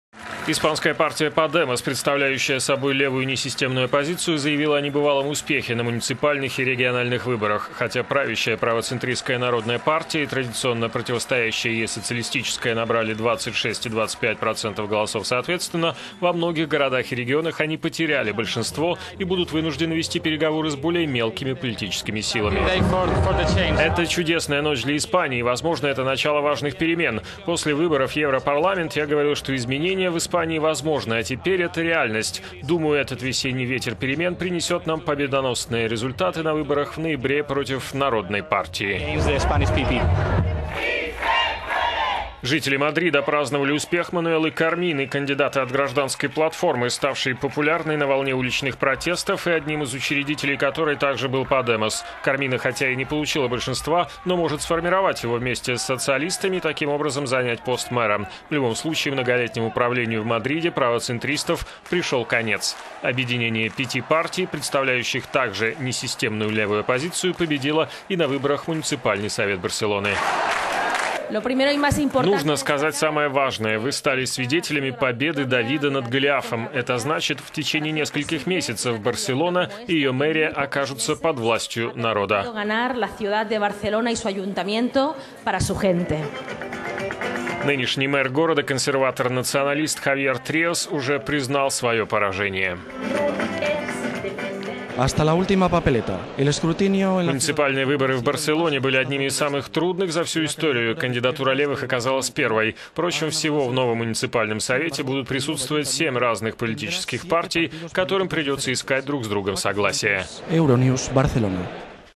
Выборы в Испании: несистемная левая оппозиция сильна как никогда
Испанская партия "Подемос", представляющая собой левую несистемную оппозицию, заявила о небывалом успехе на муниципальных и региональных выборах. Хотя правящая правоцентристская Народная партия и традиционно противостоящая ей социалистическая набрали 26 и 25 процентов голосов соответственно, во многих городах и регионах они потеряли большинство и будут вынуждены вести переговоры с более мелкими политическими силами.

Пабло Иглесиас, лидер партии "Подемос":

"Это чудесная ночь для Испании. И воз…